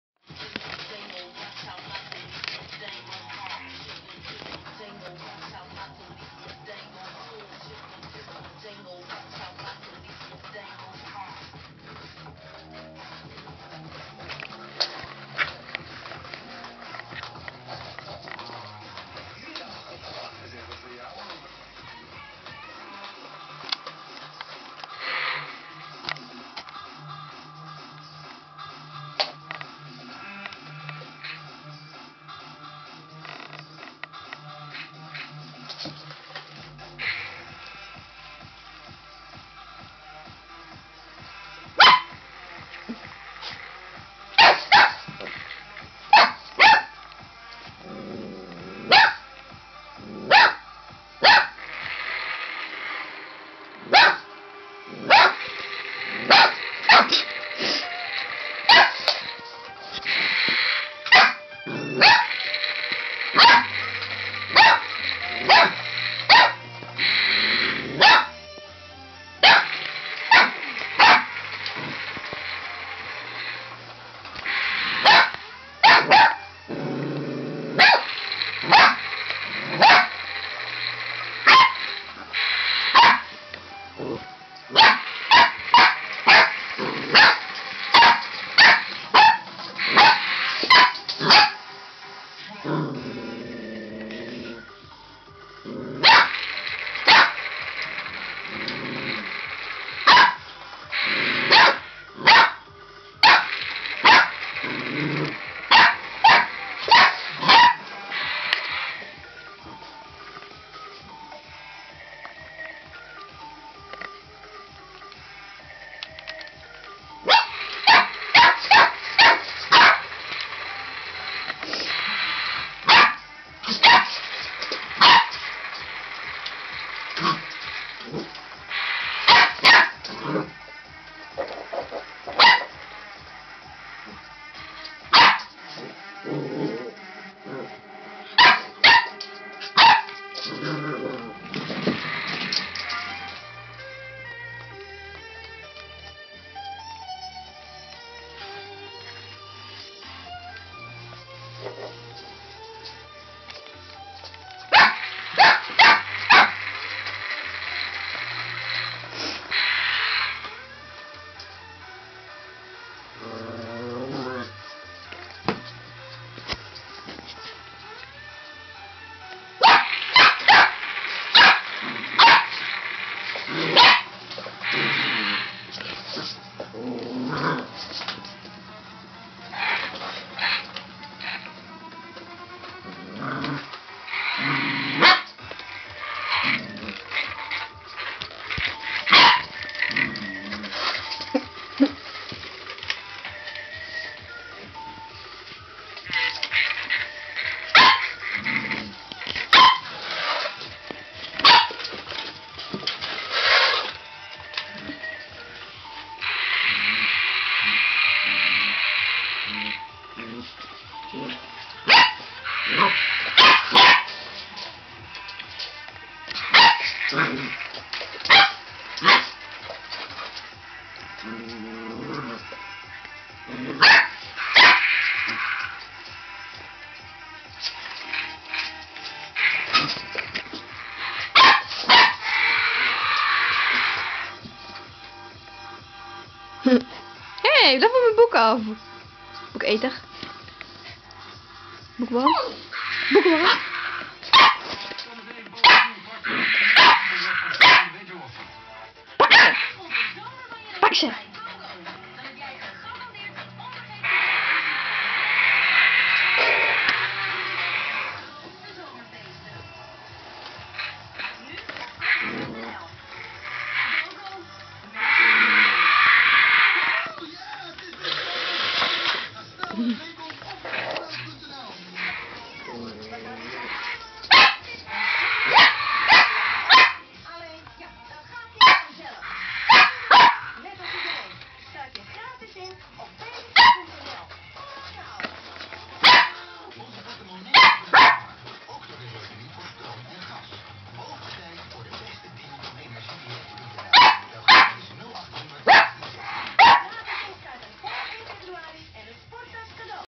funny border collie puppy and a robot dinosaure
skylar is a red merle tricolar border collie, born 18-11-13.
this channel is about here and nothing else atleast i hope so..